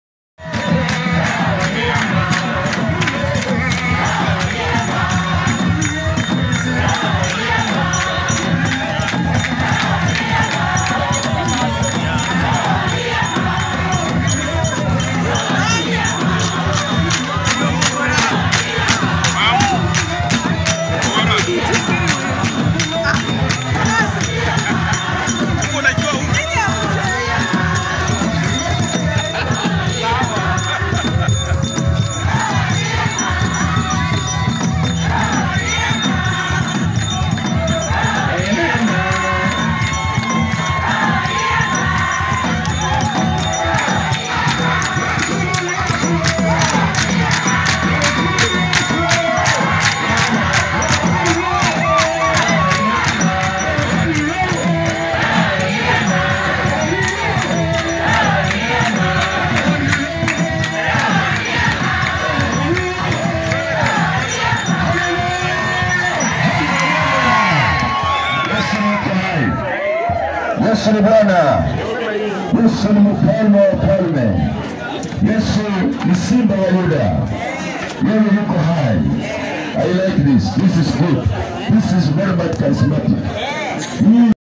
PAG Chuch when morning one of their full member at Lumakanda Lugari district
Roho ni roho roho wa ndiyama